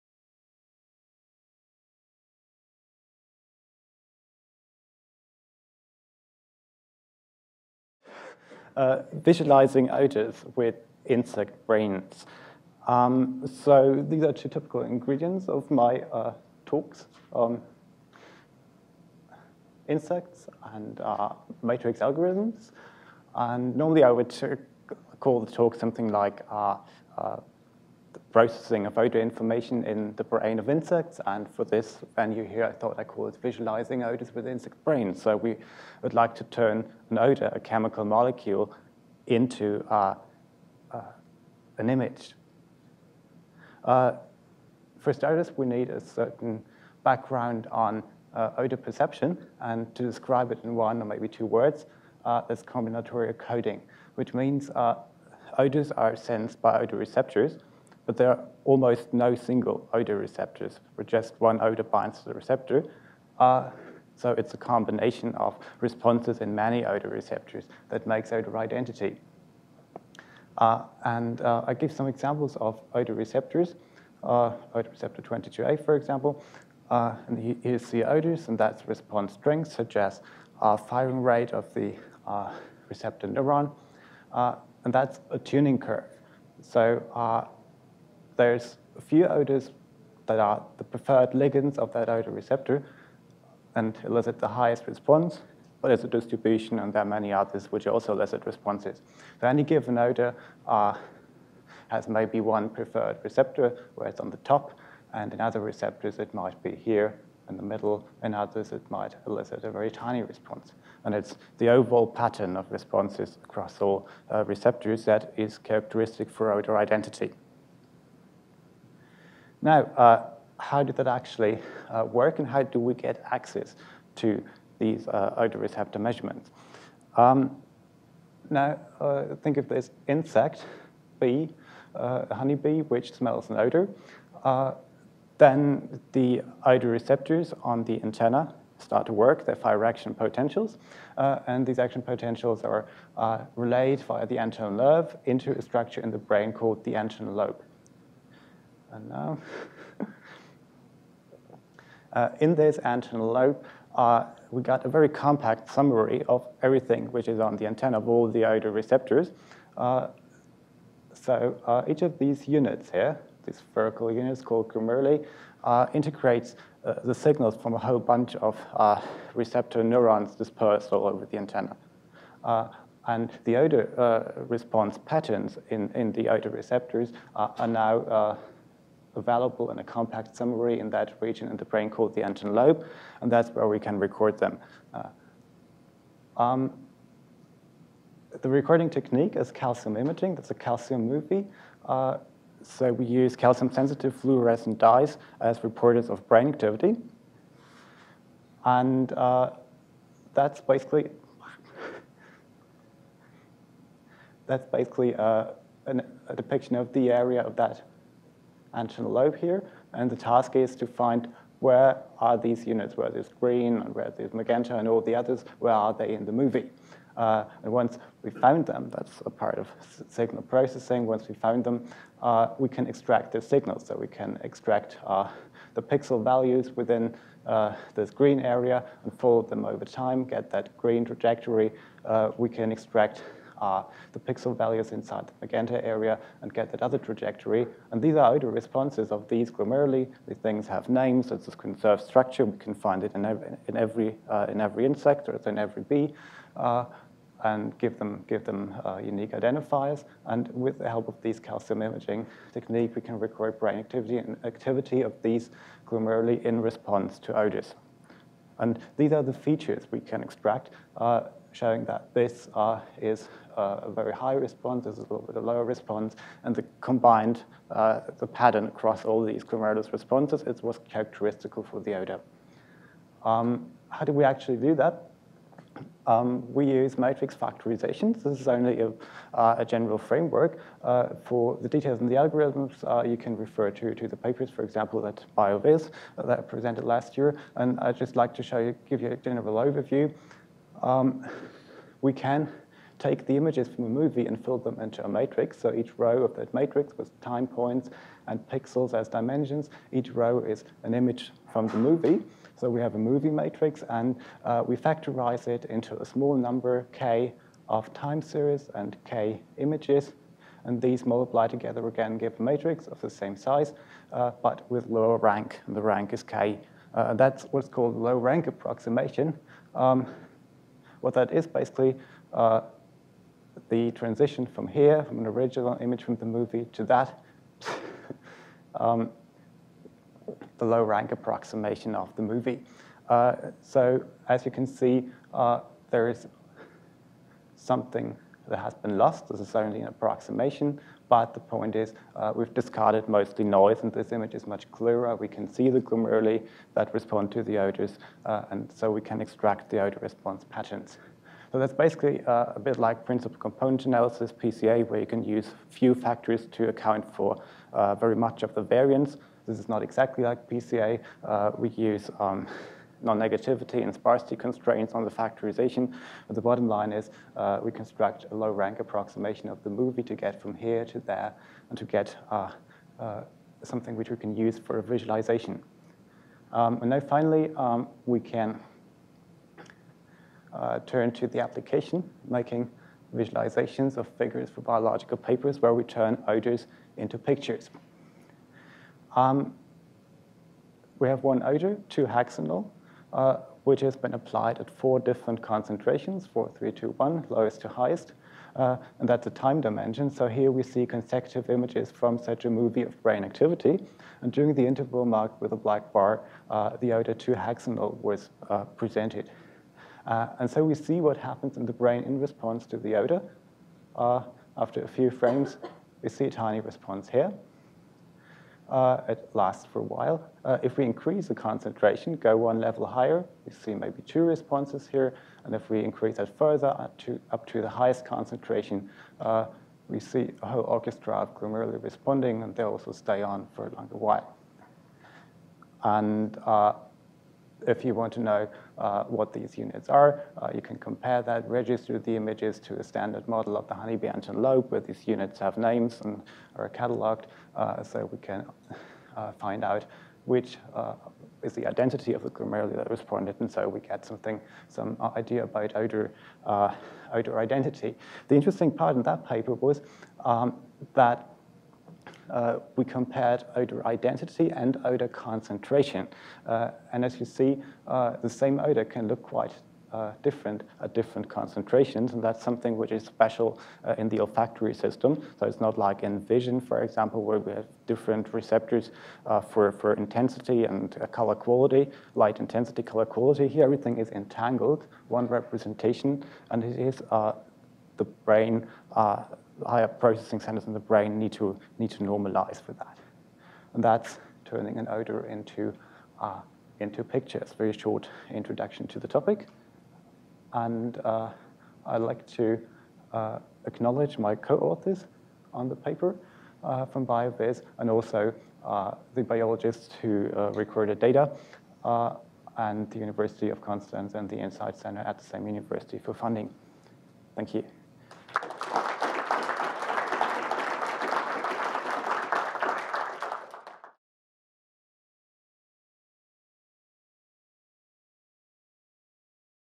VIZBI 2013: BioVis Honorable Mention - Martin Strauch
VIZBI 2013, the 4th international meeting on Visualizing Biological Data was held March 20-22, at the Broad Institute.
VIZBI 2013 brought together scientists, illustrators, and designers actively using or developing computational visualization to study a diverse range of biological data.
For information about data visualization efforts at the Broad Institute, please visit:‪

VIZBI is an international conference series on visualizing biological data (‪ funded by NIH & EMBO.